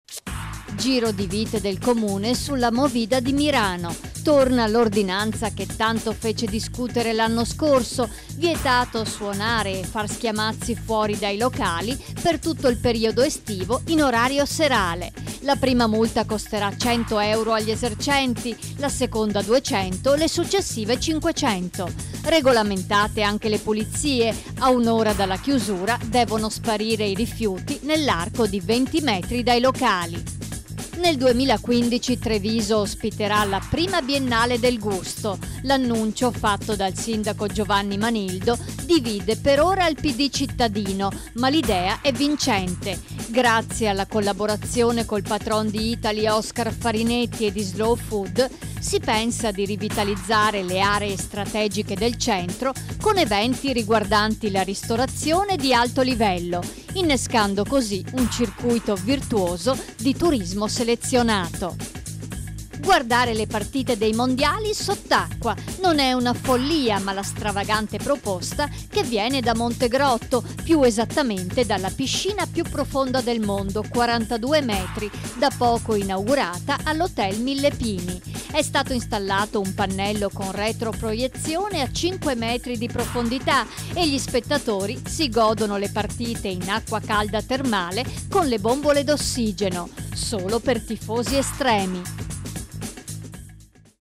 Le brevi del 17 giugno
MIRANO (VE) - Torna l'ordinanza anti-rumore
TREVISO - Biennale del gusto
MONTEGROTTO TERME (PD) - I mondiali sott'acqua